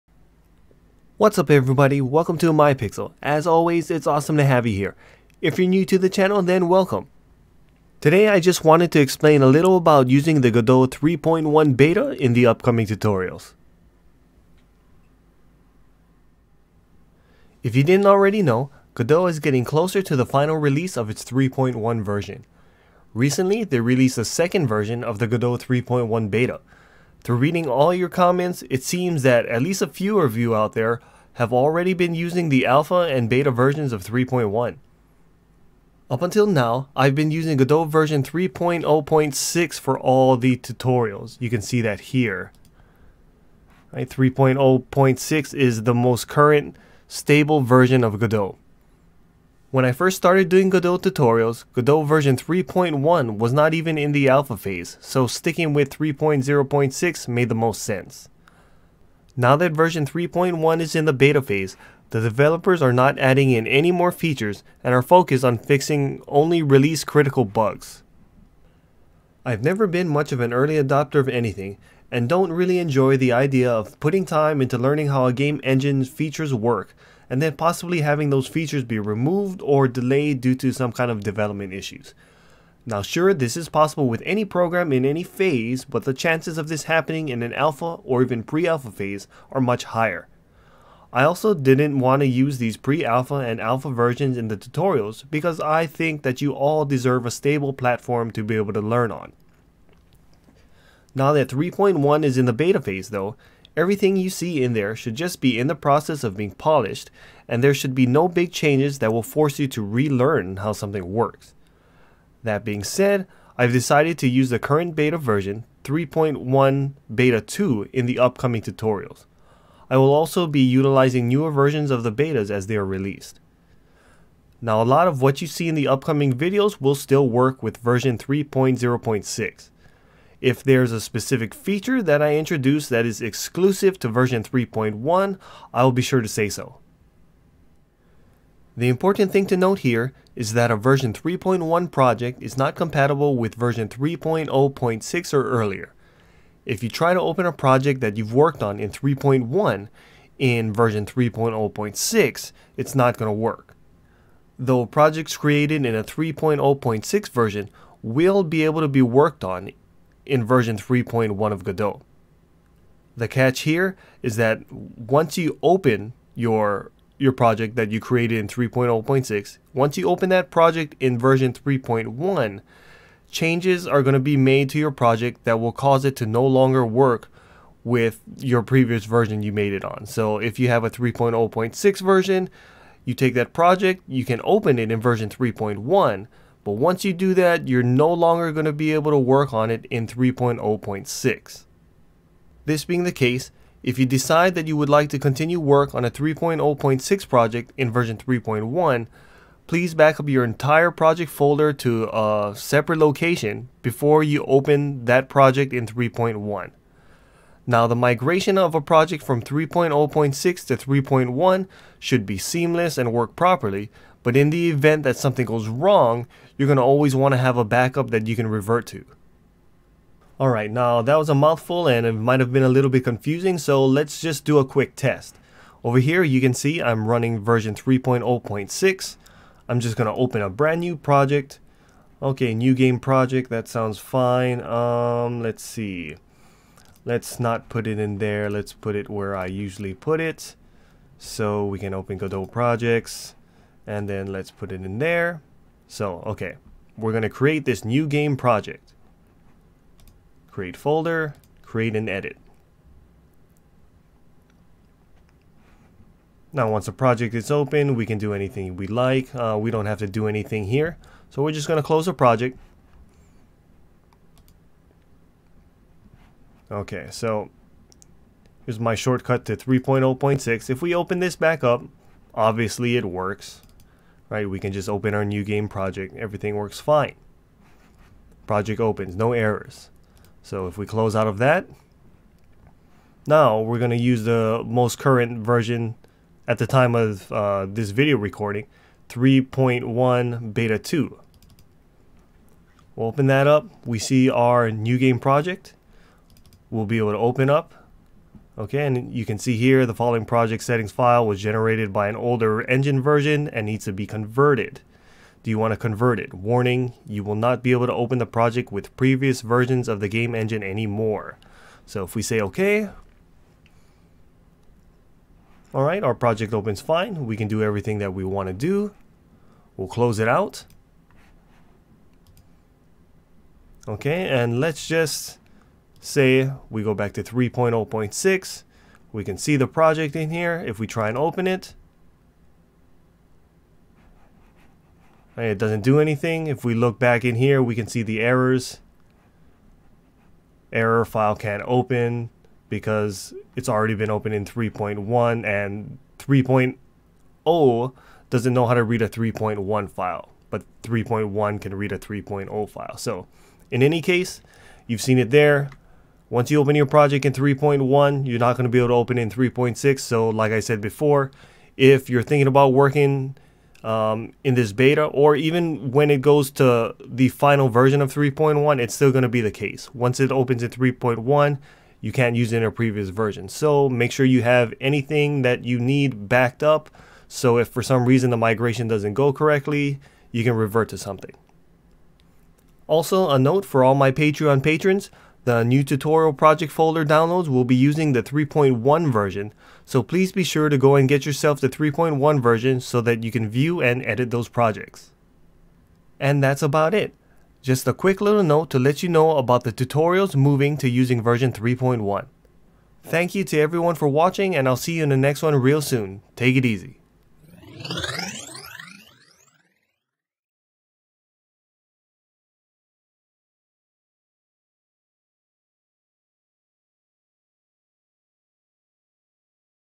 New Tutorials Using Godot 3.1 Beta
New tutorials will now be using version 3.1. Here's a little info about it =)